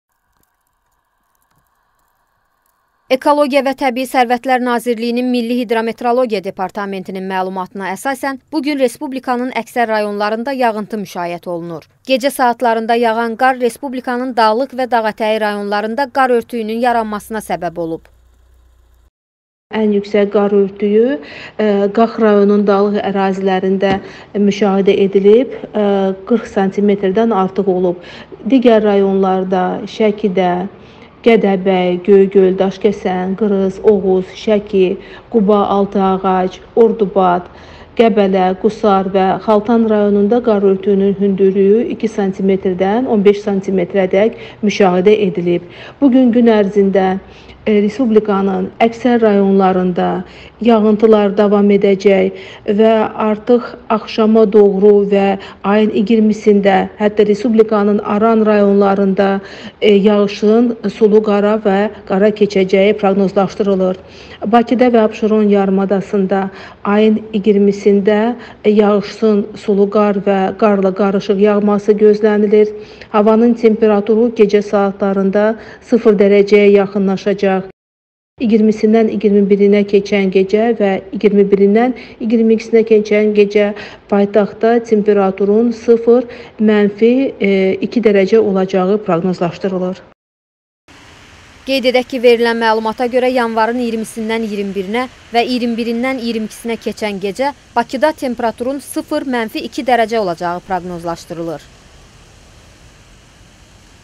Qaxda 40 sm-lik qar örtüyü
Bakıda havanın temperaturunun 0 dərəcə olacağı proqnozlaşdırılır